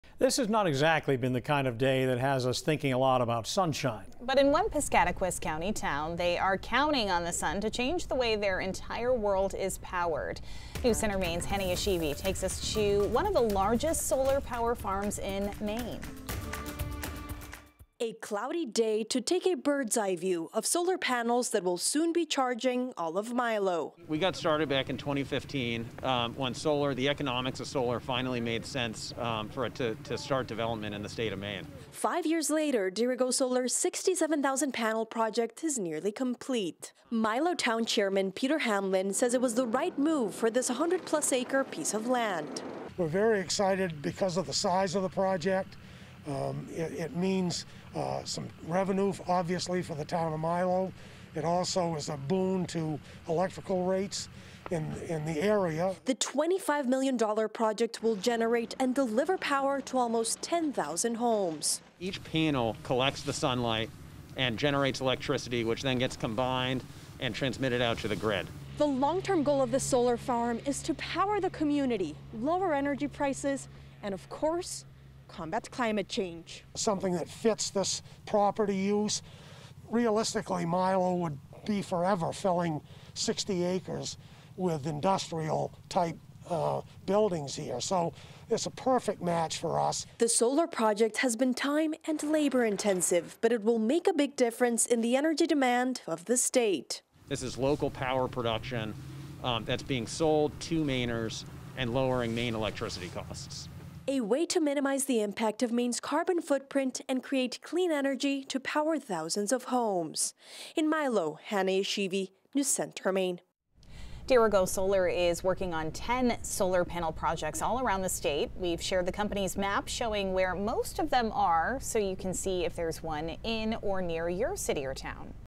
Milo Solar Farm to power 10,000 homes
Dirigo Solar's 67-thousand solar panel project is nearly complete and will generate power to almost 10 thousand homes.